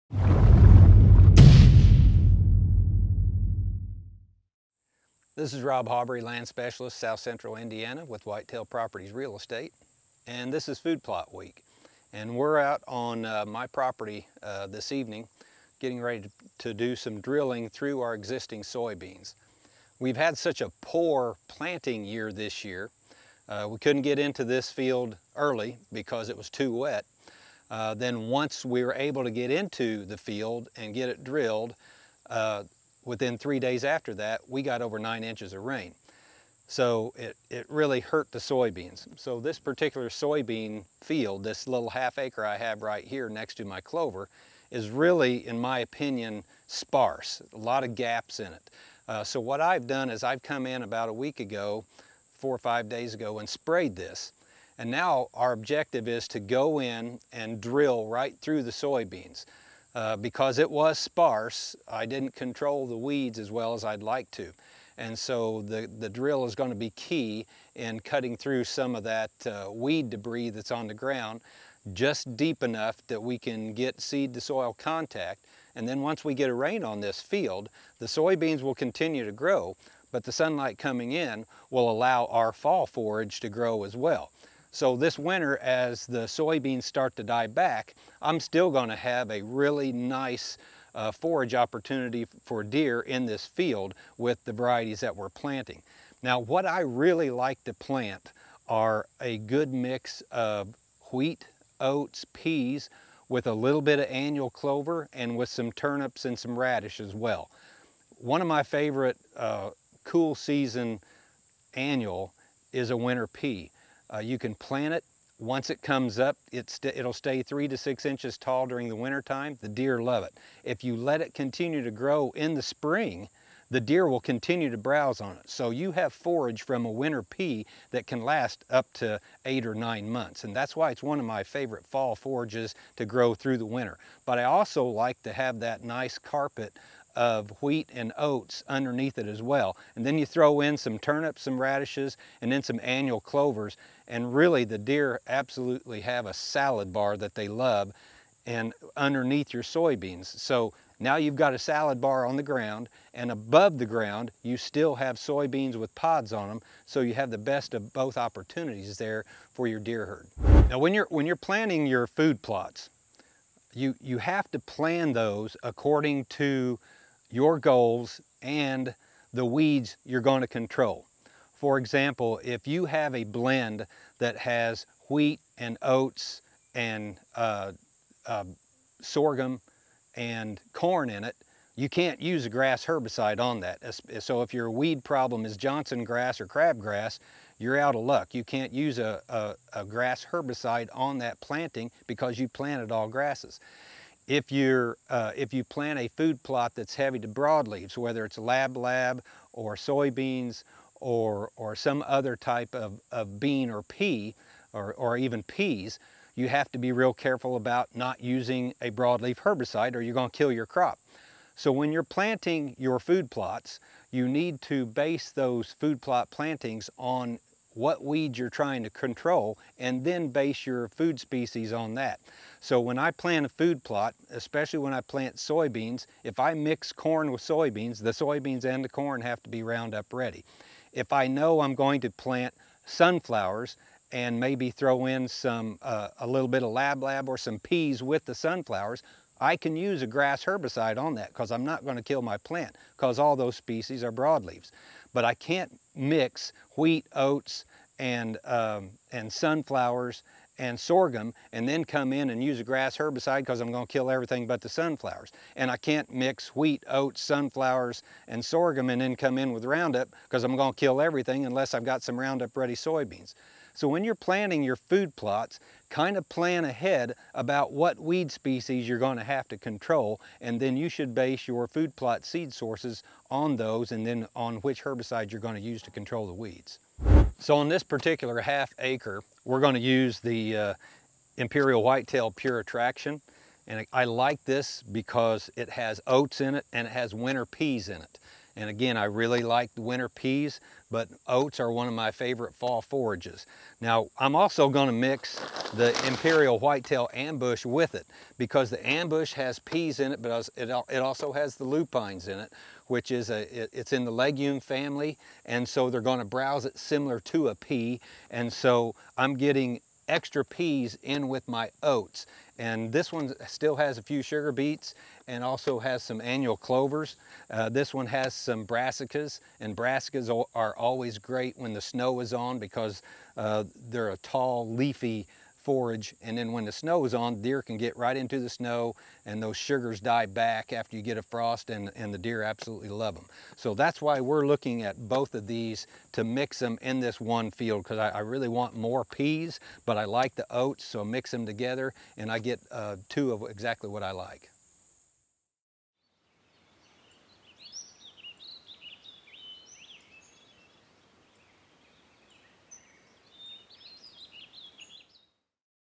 FALL FOOD PLOTS | Seed Combinations and Strategies
Now is the time to start getting your fall food plots in the ground! Weather you are establishing a new food plot or adding forage to an already established plot there are many things to consider. Today we are salvaging a soybean food plot that did quite produce because of tough growing season conditions. Choosing the right seed combinations is key in having a successful plot this fall!